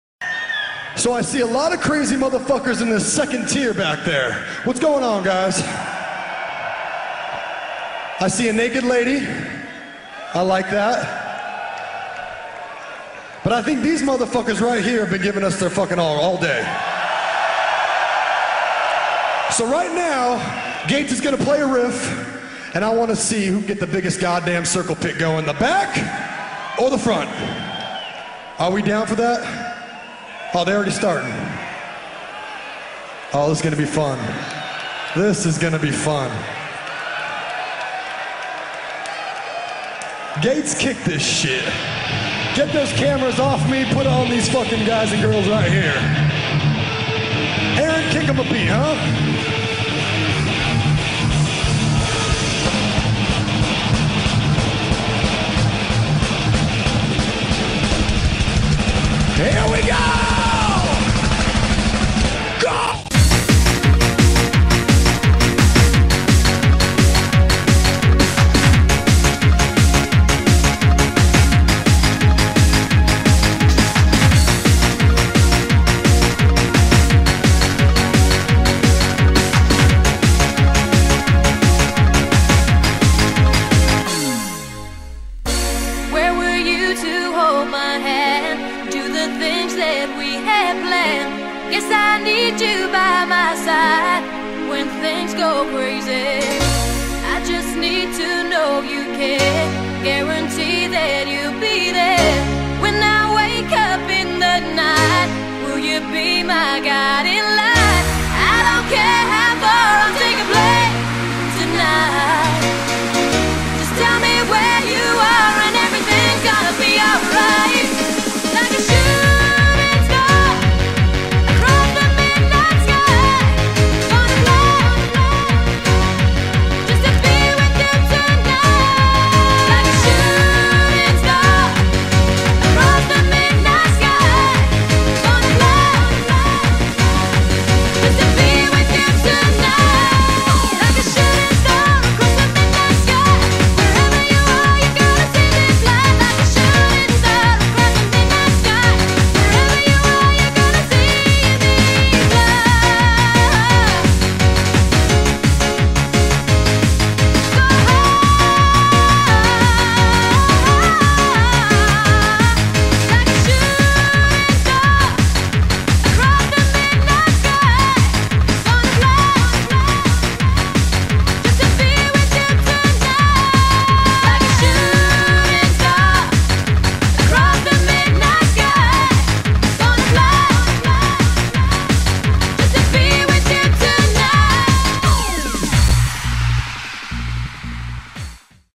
Happy Hardcore Metal Moshpit
Ever wonder what a moshpit would look like with Happy Hardcore music instead of metal? So did I.